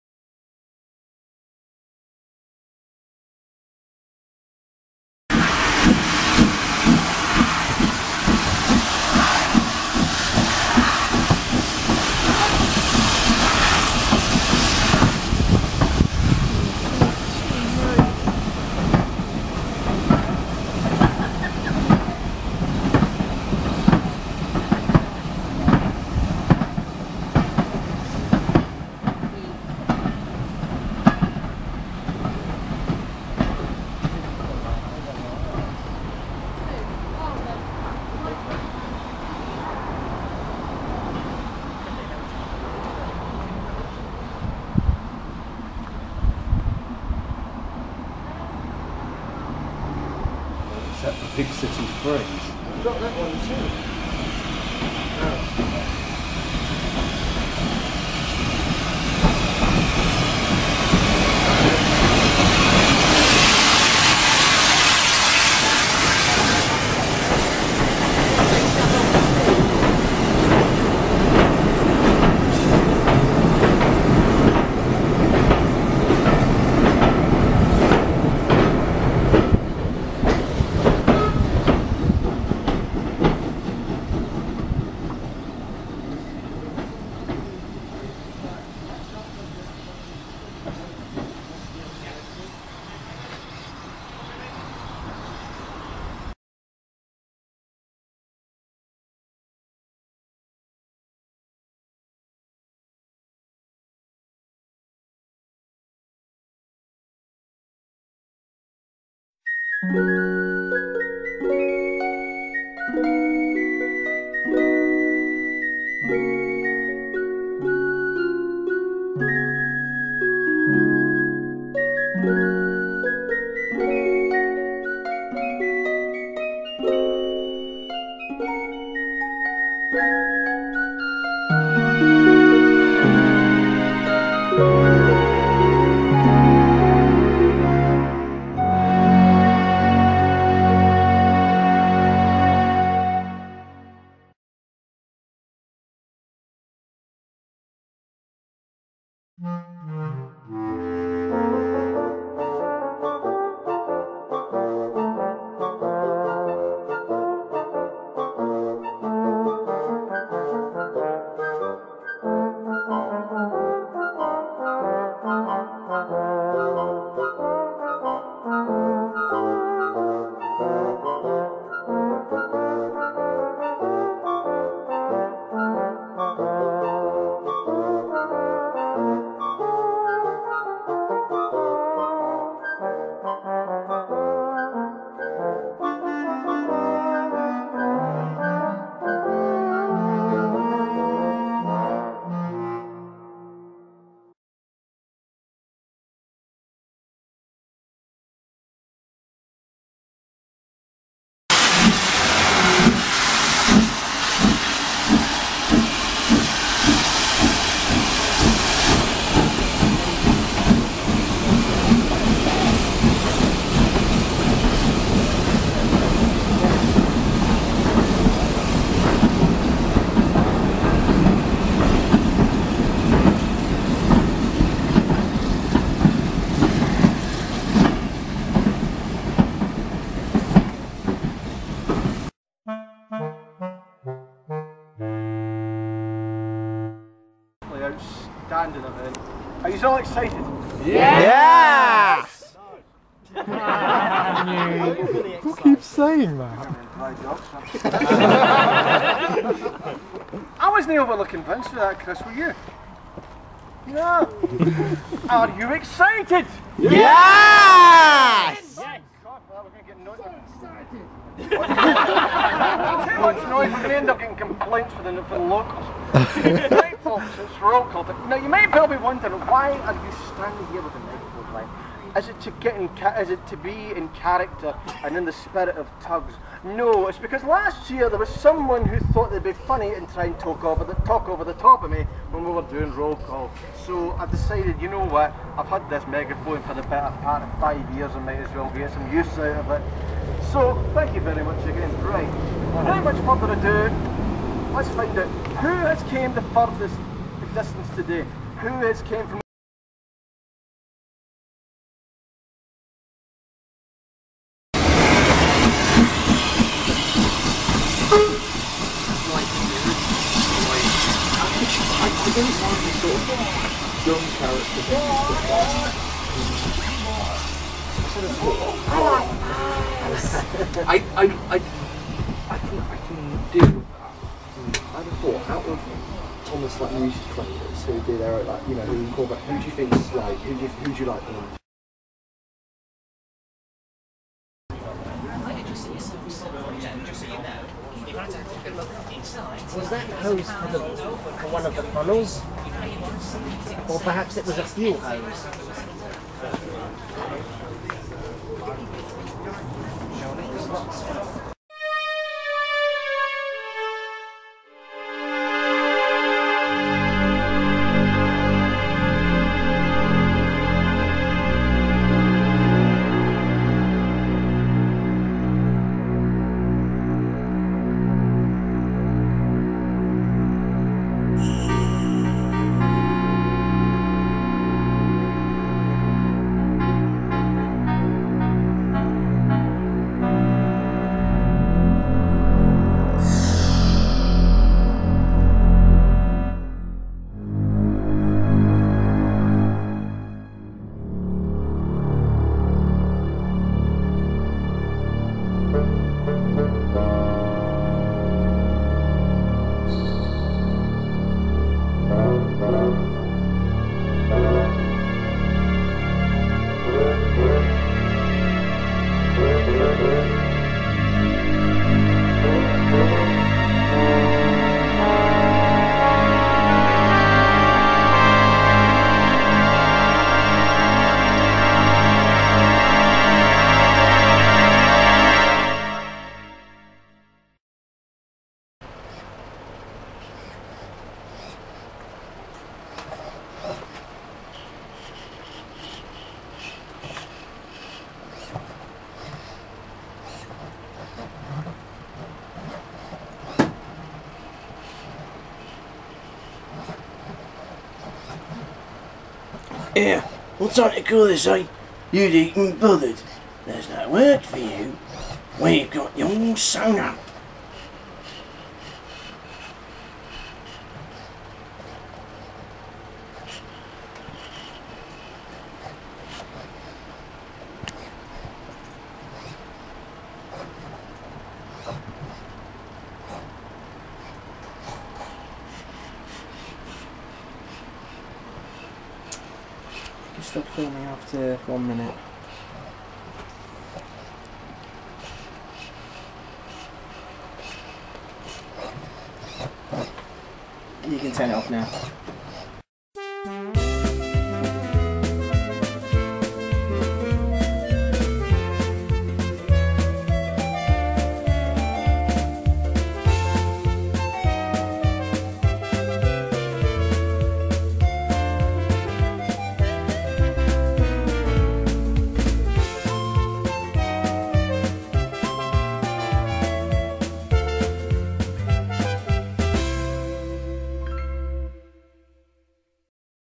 My Trip to the Bigg Weekend at the Midland Railway August 2019
This video is my trip to the Bigg Weekend that was held at the Midland Railway, Butterely Station at Derby. I took some great pictures and did some filming as well. I had a fun time seeing everyone there again and the event, I think, was a fantastic success. I hope everyone else enjoyed themselves too.

Footage filmed by me and Elliot Killick-Ward.